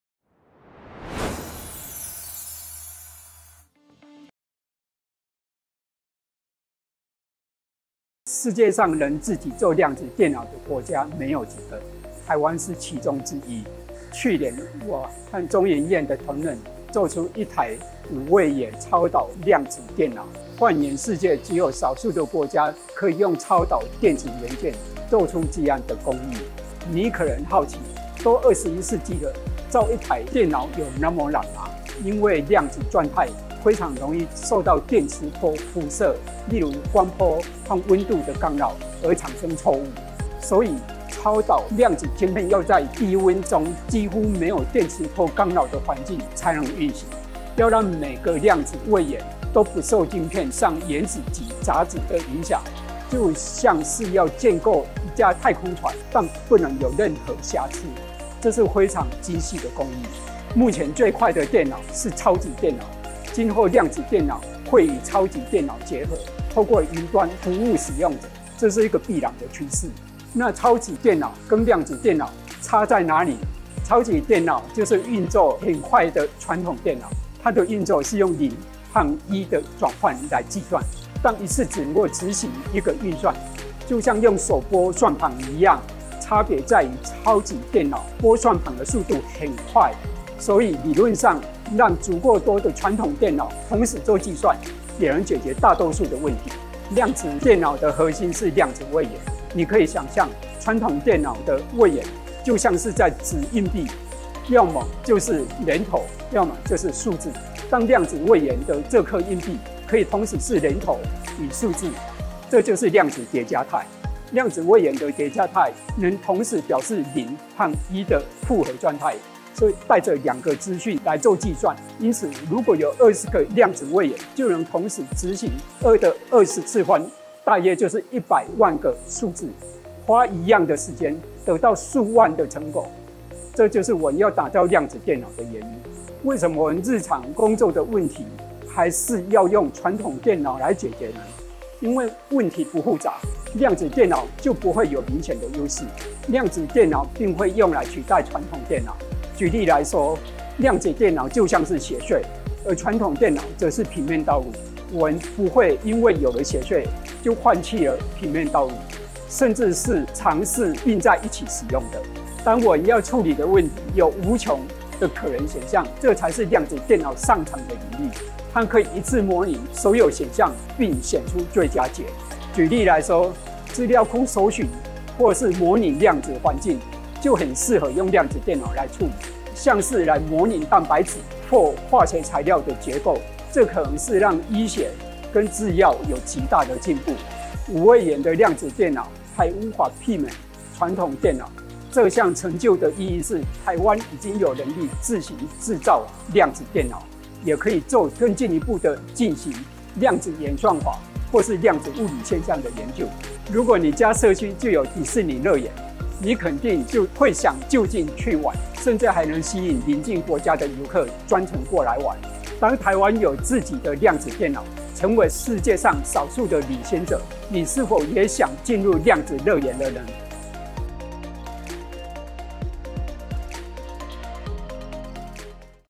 【專家領進門】只有幾個國家做得出來?! 臺灣第一台 5 位元超導量子電腦
量子電腦跟傳統電腦有什麼不一樣？它會取代傳統電腦嗎？中央研究院物理研究所陳啟東研究員帶你開箱臺灣自製的 5 位元超導量子電腦，並介紹「量子國家隊」的下一步！